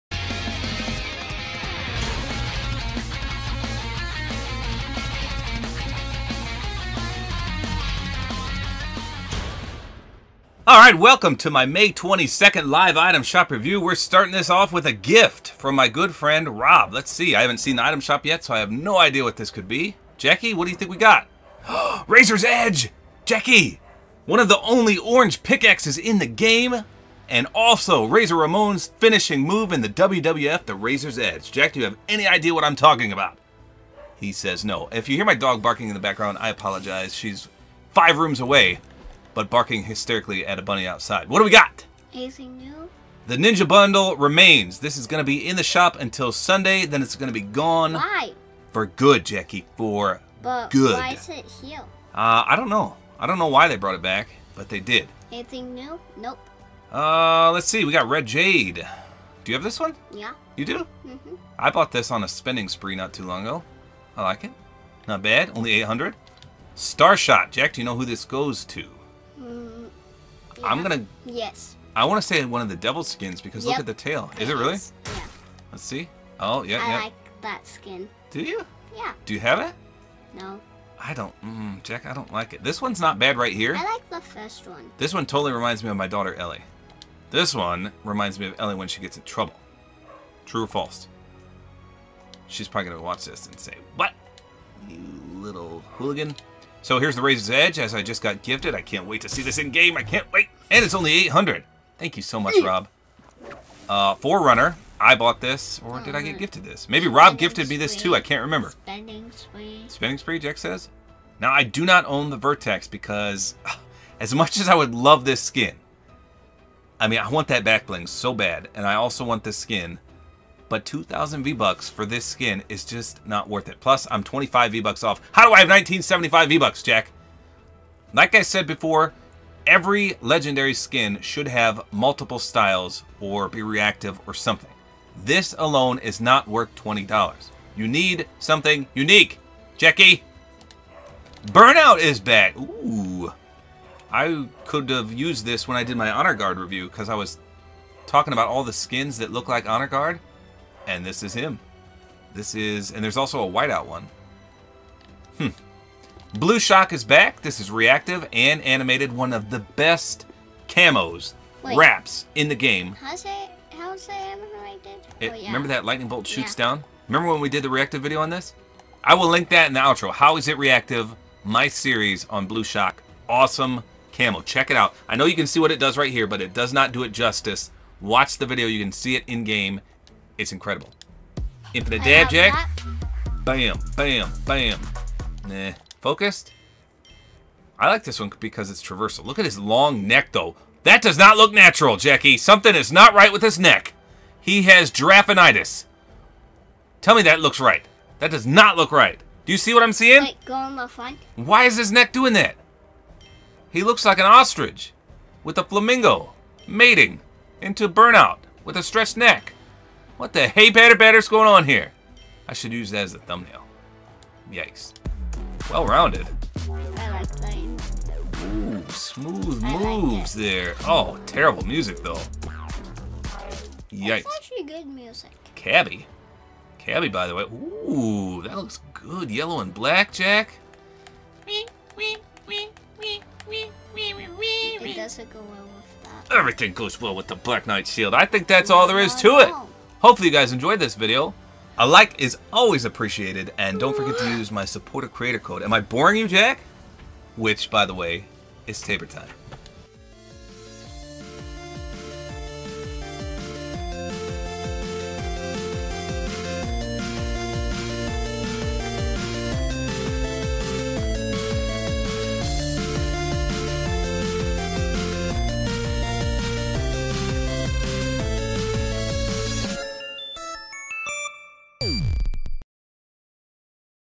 THAT NECK, THOUGH
Holy long neck!  This is the item shop review for 05/22/20.